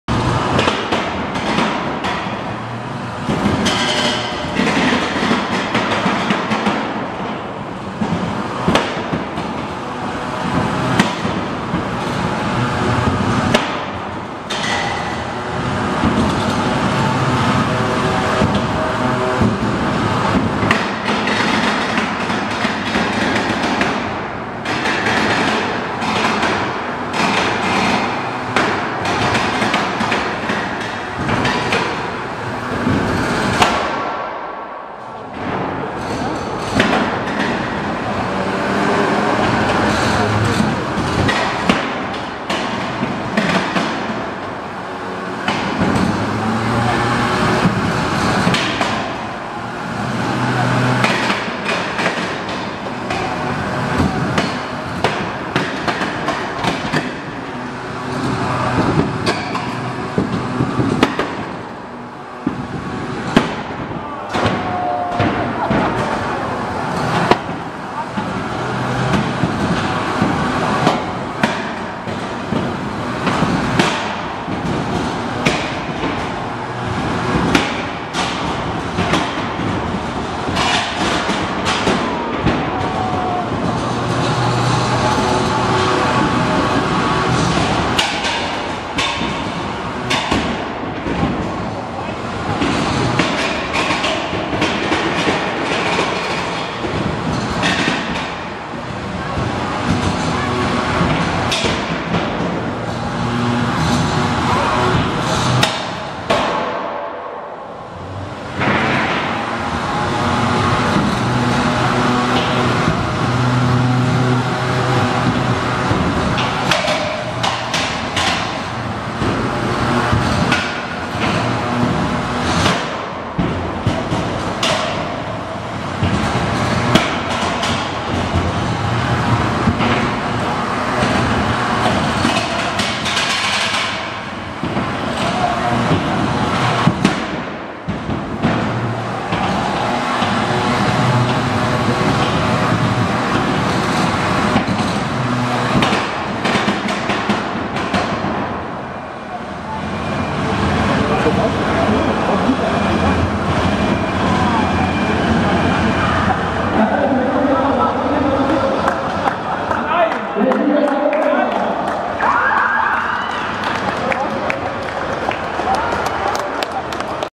MMMv.23.Featherweight.Finale.Omega.vs.Equinox
Kampf der Featherweight Klasse (13,6Kg) bei der INTERMODELLBAU 2018 in Dortmund, mehr Infos über unseren Verein der German Roboteers Association e.V. in unserem Forum